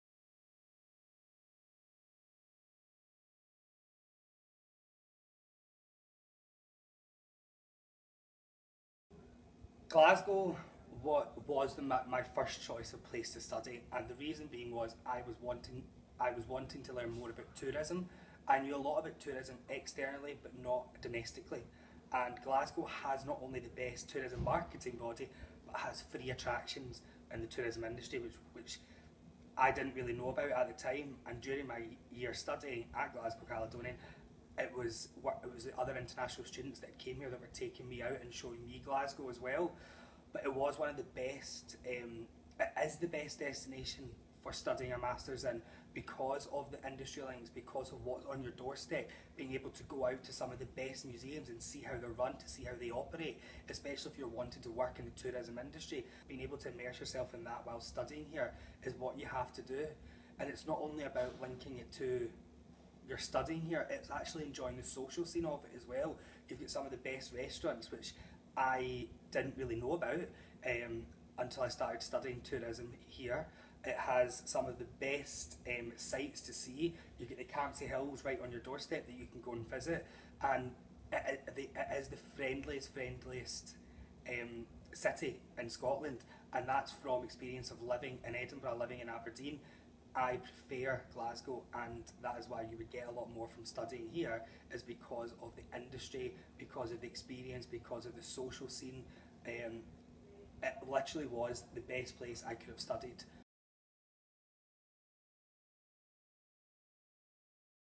Daniel Baxter, "Glasgow and Scotland" - MSc International Tourism Management
Find out more about our MSc International Tourism Management programme

Daniel discusses what it is like to live and study in Glasgow and Scotland, and the Tourism opportunities that Glasgow provides.